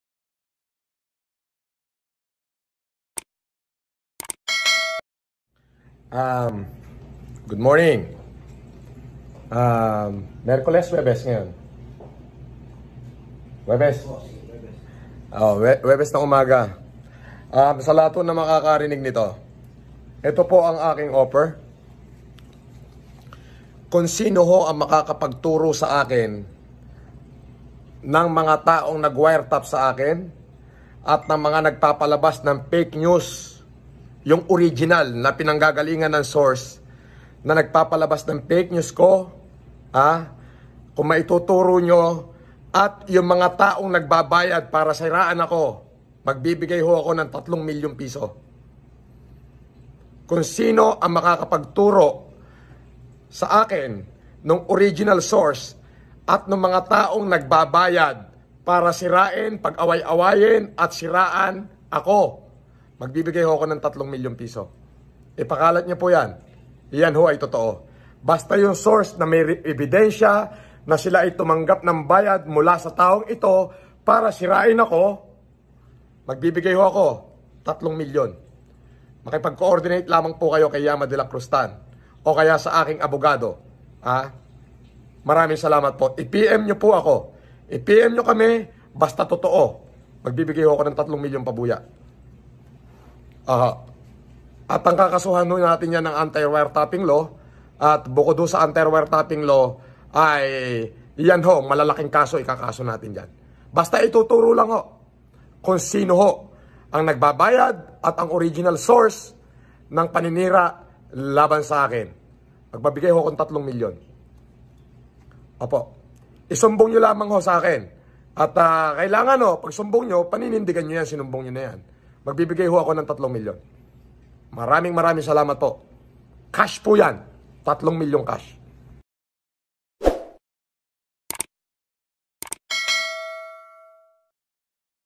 FRANCIS LEO MARCOS 3 MILLION REWARD PARA SA MAKAPAG TURO NG FINANCER SA MGA BASHER NI SIR FLM
Breaking news Sir FLM handang mag bigay ng 3 million kung sino man ang makapag turo sa financer ng kanyang mga basher